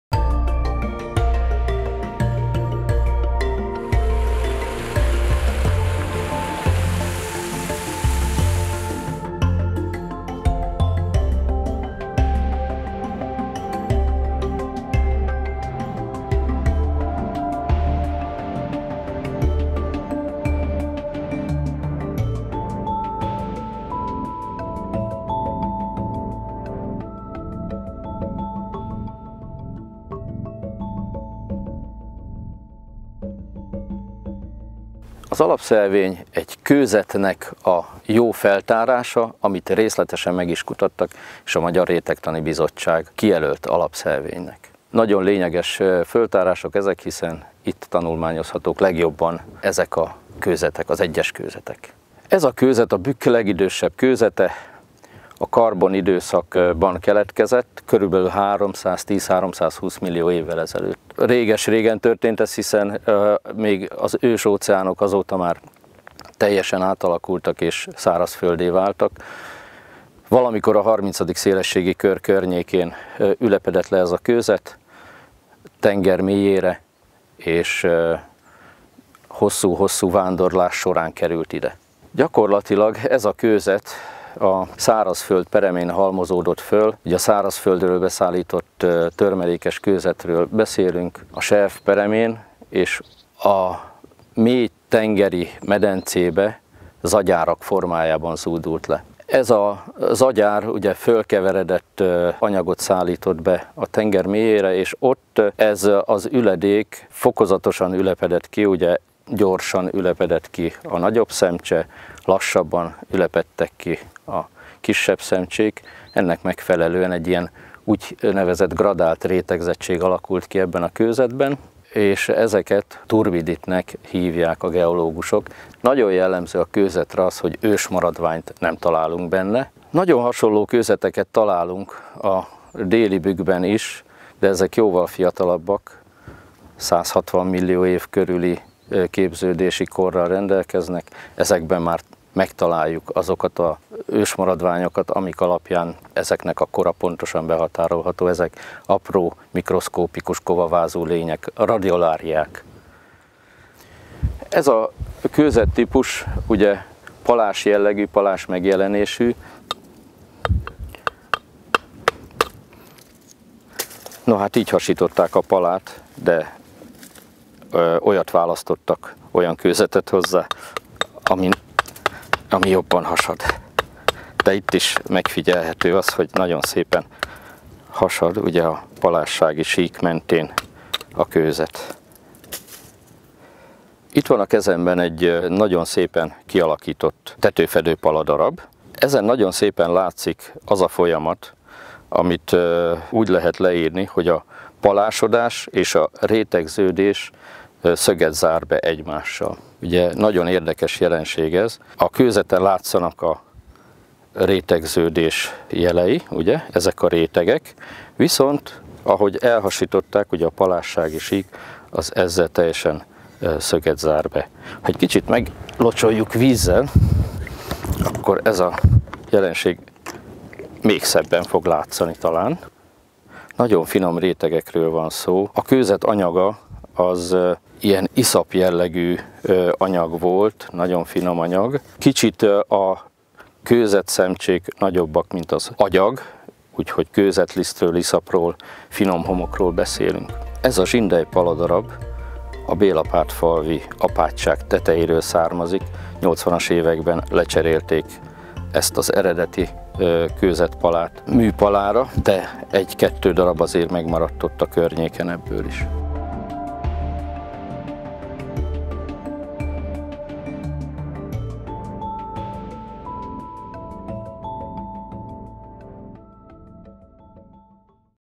Bükk-vidék Geopark: Szilvásváradi formáció
A sorozat a Bükk-vidék Geopark geosite-jait mutatja be.
Bővebb info: bnpi.hu

A XXIX. Teleki Pál Kárpát-medencei földrajz-földtan verseny online feladatának segédanyaga.
Készítette: Holló Sándor, Kozma Attila
Bükki Nemzeti Park Igazgatóság, 2020